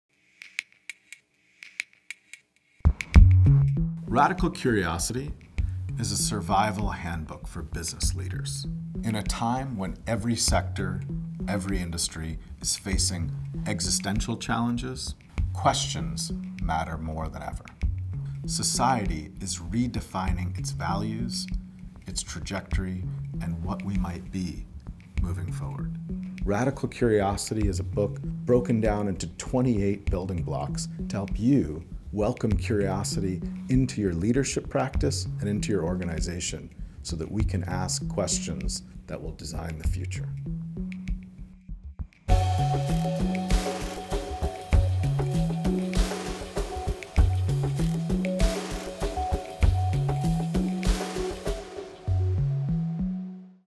Seth Goldenberg on Radical Curiosity | PRH Speakers Bureau
Why is radical curiosity crucial to the success of your business? PRH speaker Seth Goldenberg breaks down how to welcome curiosity into your leadership and organization.